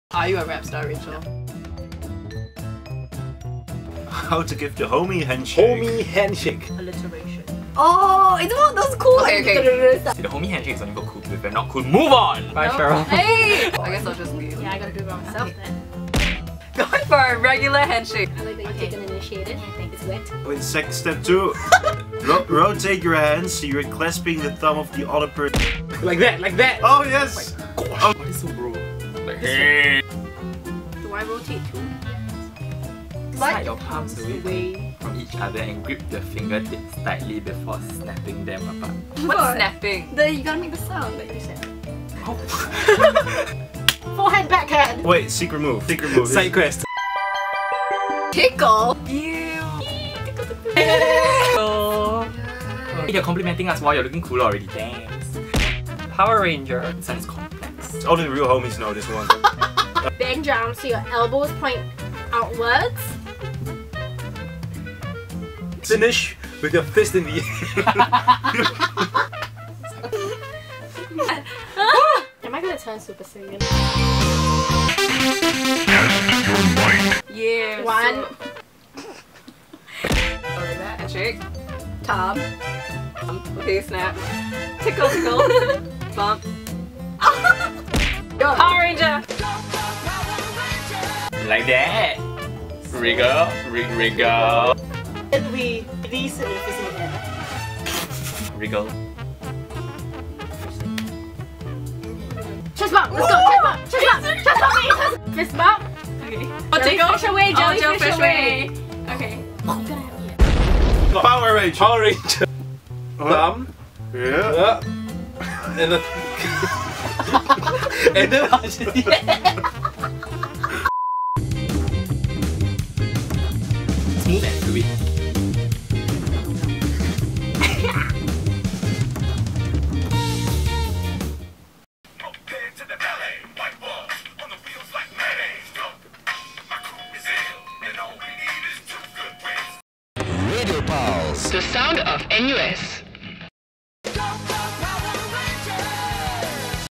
Broadcasters Learn The Homie Handshake
Watch as WikiHow teaches our broadcasters the essential life skill of how to properly greet the homie!

You can also check out our Instagram and Twitter account with the following handle: @radiopulze